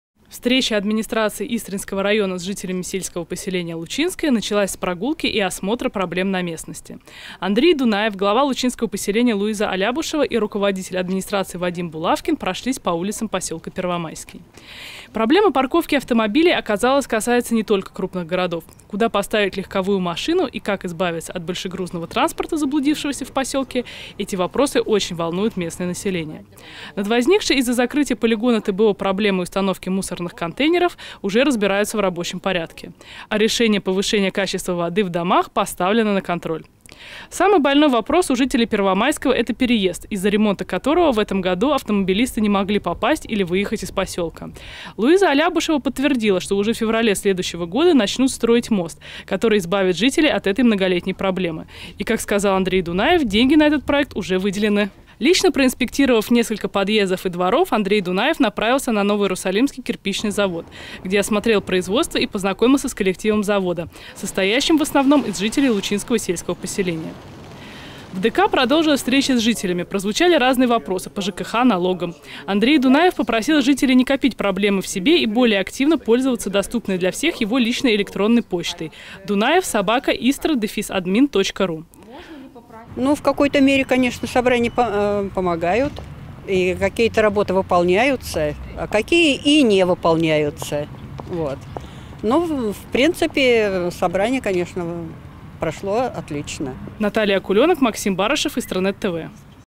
Андрей Дунаев встретился с жителями в Лучинском сельском поселении
Руководитель администрации Истринского района встретился с жителями Лучинского сельского поселения.